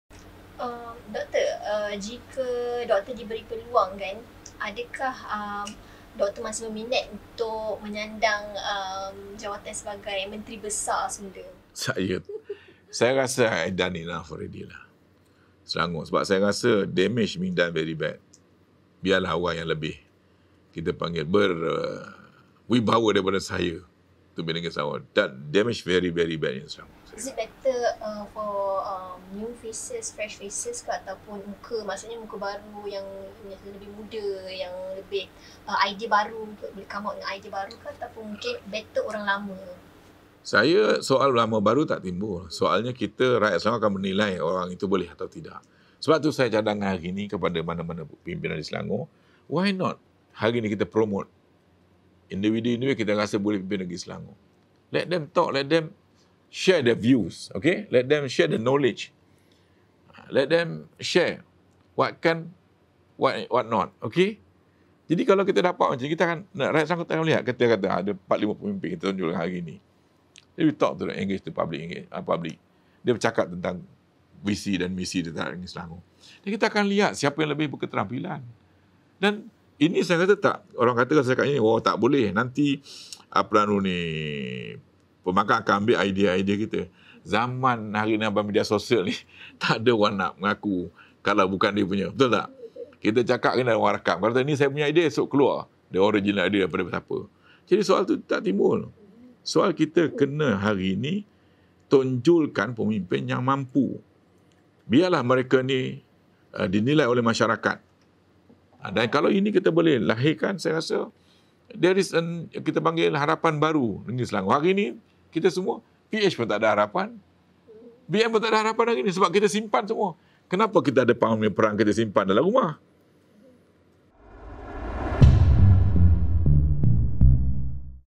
Khir Toyo: Let someone more capable than me be Selangor MB
Former Selangor Menteri Besar Dr Mohamad Khir Toyo said it would be better for someone with a better vision for Selangor to be given the opportunity to be BN's Selangor MB candidate.

He said this when asked whether he would like a second chance to be Selangor menteri besar in an interview with Malaysiakini this week.

“I’ve done enough already. I think the damage done is very bad, let it be someone who is more capable than I am.

“The question of whether it is the old guard or new blood does not arise… The people of Selangor will judge whether the person can lead or not,” he said.